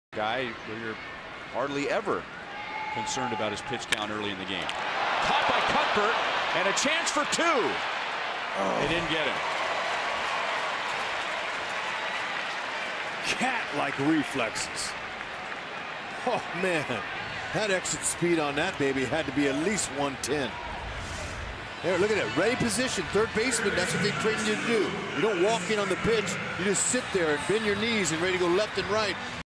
KC@HOU: Cuthbert grabs a line drive
Cheslor Cuthbert makes a great play and snags a sharp line drive down the third-base line in the bottom of the 5th inning